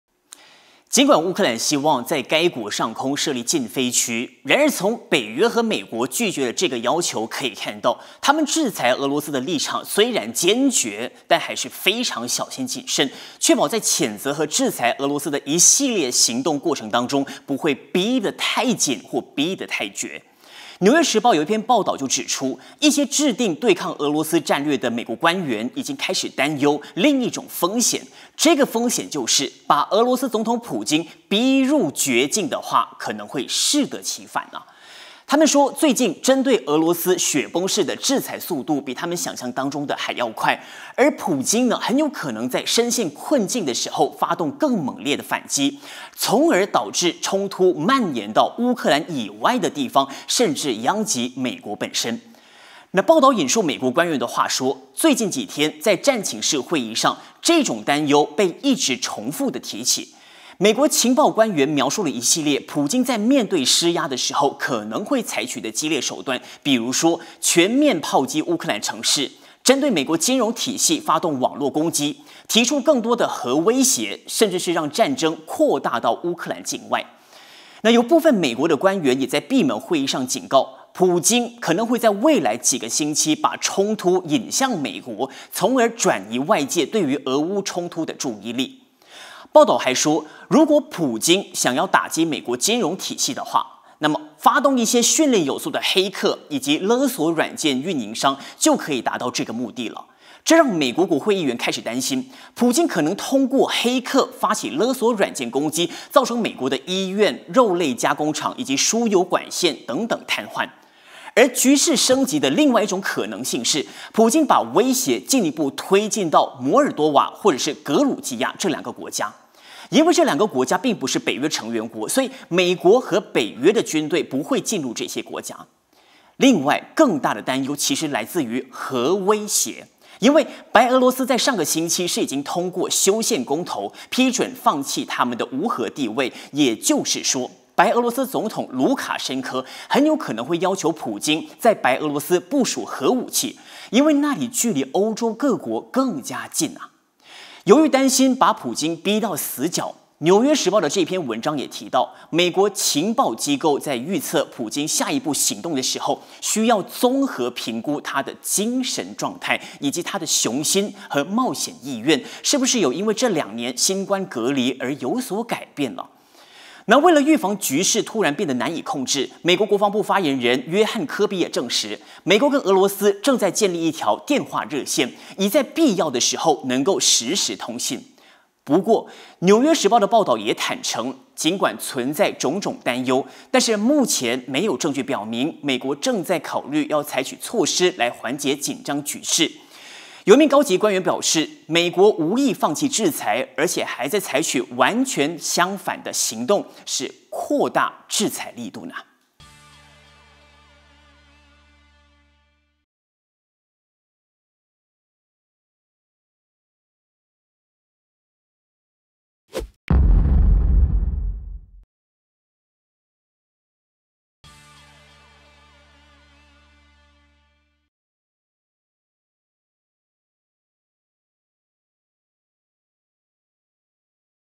雪崩式制裁俄罗斯 普京或以更激烈手段反击？ | 八点最热报 05/03/2022
《纽约时报》一篇报道指出，针对俄罗斯雪崩式的制裁速度，俄罗斯总统普京在未来很大可能会发动更猛烈的反击，把冲突蔓延到乌克兰以外的地方，甚至殃及美国。